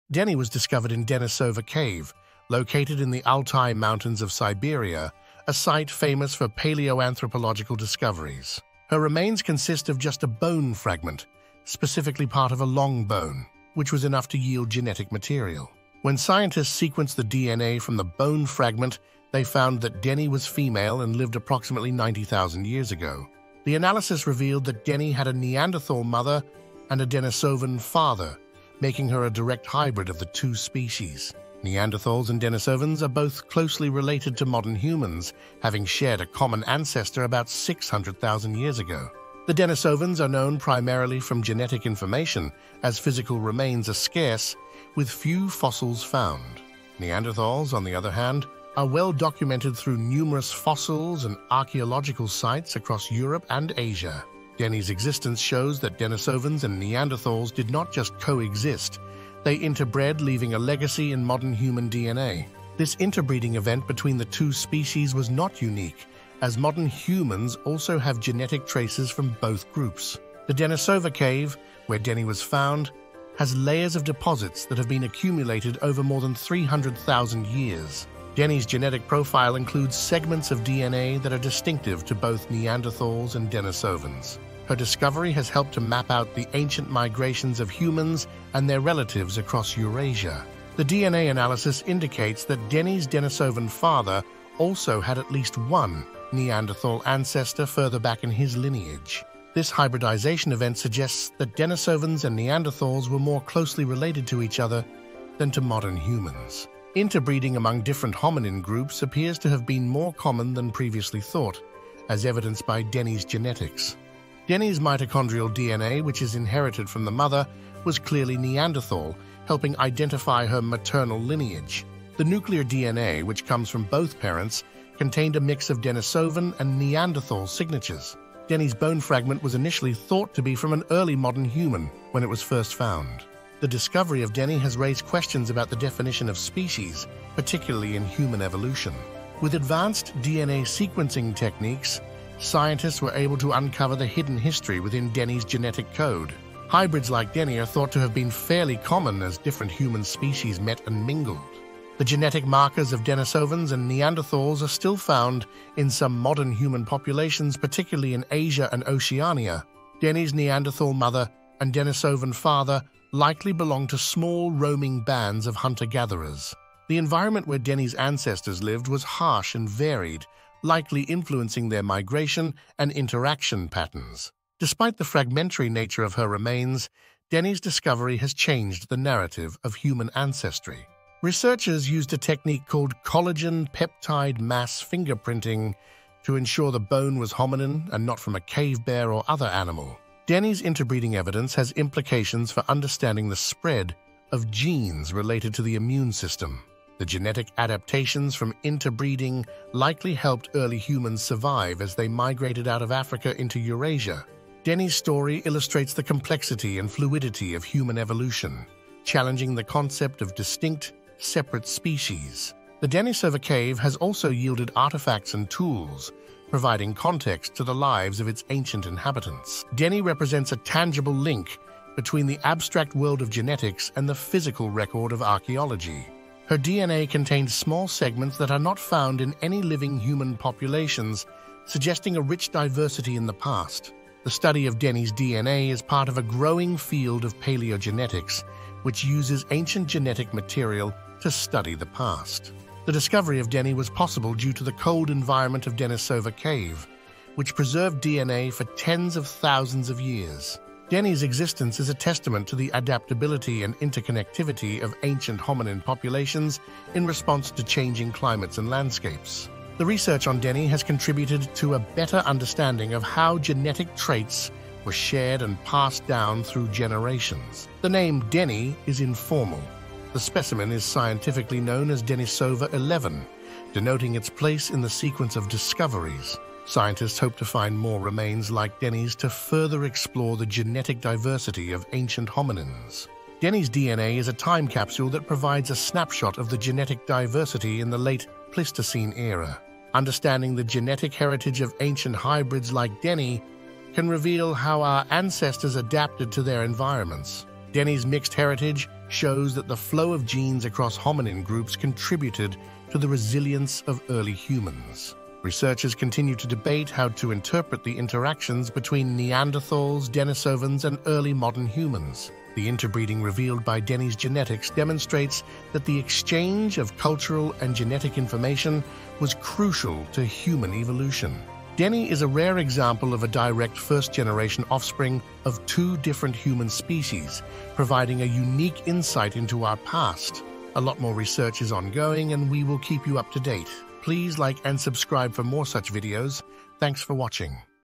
Half Neanderthal, Half Denisovan | Denny, The Only Known Inter Species Love Child
In this video we look at Denny, the only known inter species love child with a Neanderthal Mother and Denisovan father, two distinct groups of ancient humans.Denny lived around 90,000 years ago, and her existence provides clear evidence of interactions between Neanderthals and Denisovans.Modern humans also interacted with various other species in particular the neanderthals.